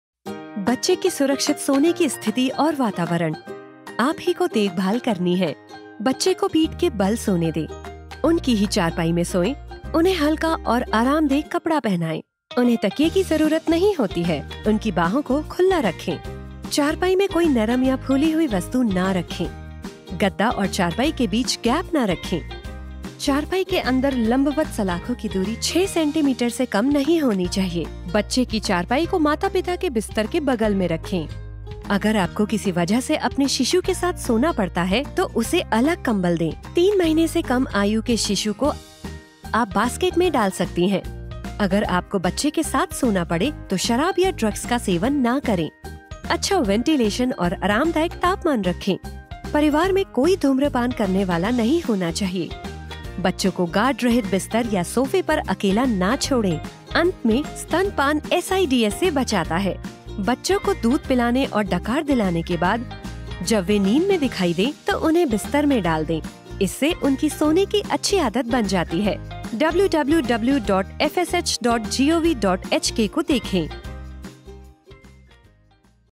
Baby's safe sleeping position and environment you are the one to care (Hindi - हिन्दी)
एनीमेशन वीडियो शिशुओं की नींद की सुरक्षा पर व्यावहारिक जानकारी प्रदान करता है, ताकि सुरक्षित शिशु नींद की प्रथाओं के महत्व के बारे में अपेक्षित माता-पिता, नए माता-पिता और आम जनता के बीच जागरूकता बढ़ाई जा सके।